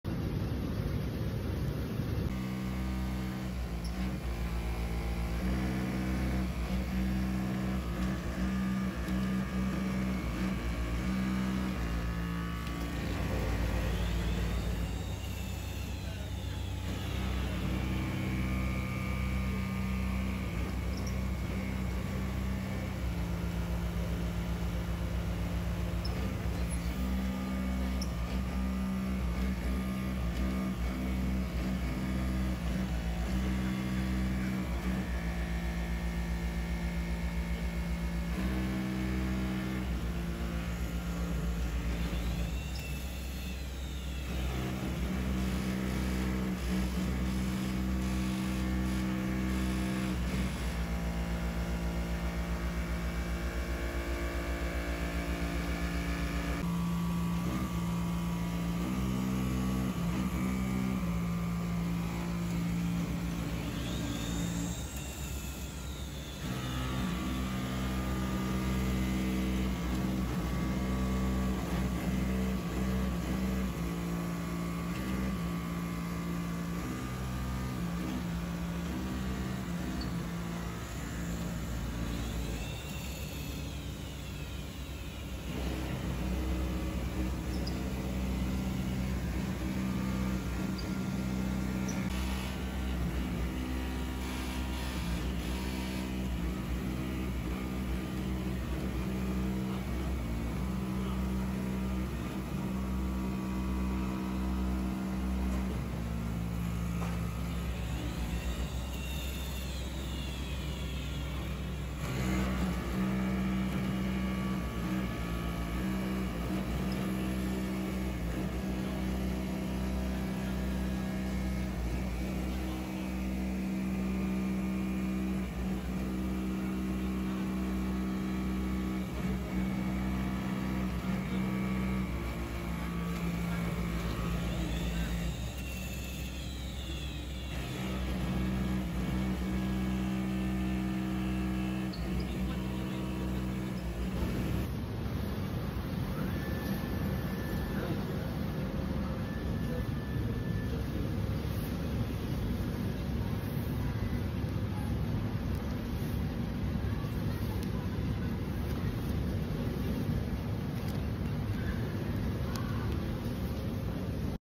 Multi Layers Fabric Garment Cutting Machine
Multi-layer cutting machine cuts clothing fabric. The fabric cutting machine is a technology widely used in the clothing industry to replace the early manual electric push knife. The garment cutting machine can improve cutting efficiency, make the cutting process safer, save materials, and improve material utilization.